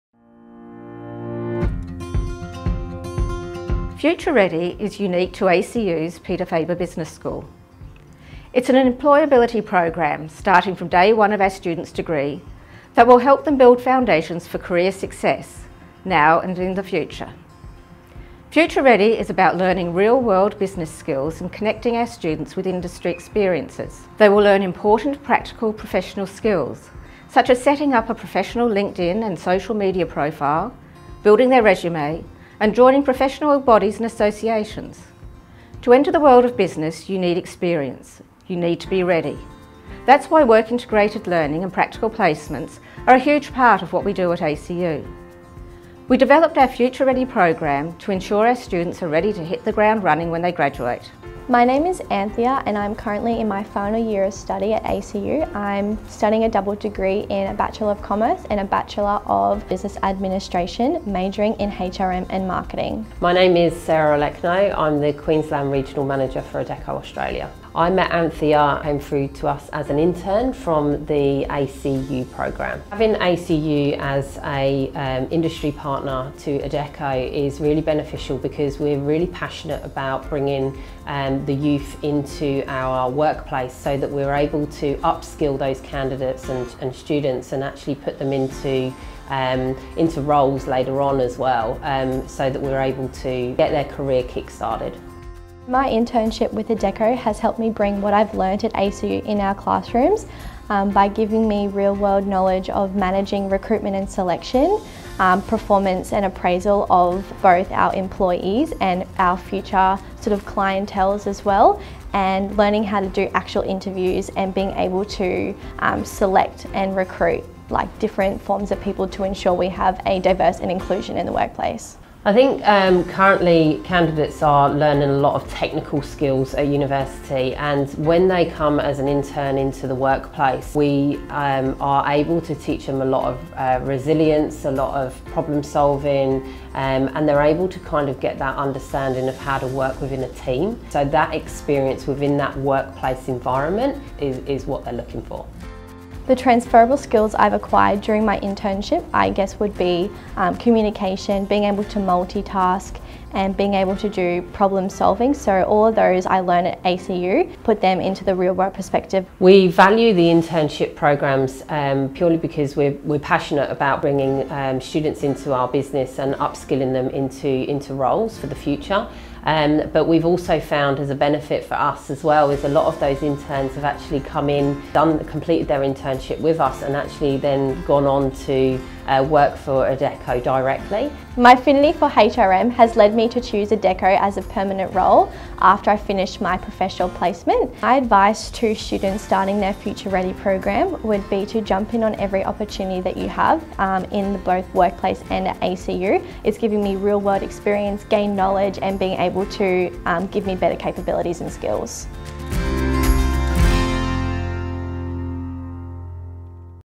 ACU Peter Faber Business School Future Ready program
When you study business and IT at ACU, you’ll graduate ready to launch your career. Our Future Ready program is designed to build each of the elements of your personal brand: your professional identity; experience; job-ready skills; and networks.